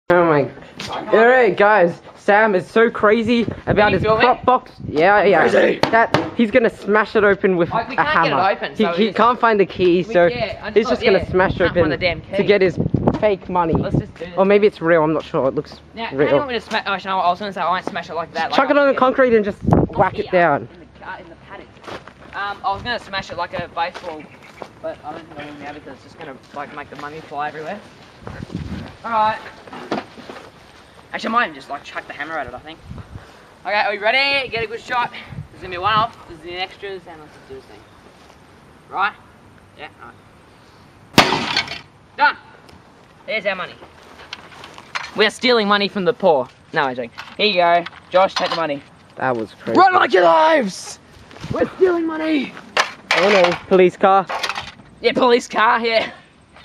Box Smash
We tried to get our prop money out of a box, but we couldn't find the key, so the only thing left to do was to smash it open with a hammer!?!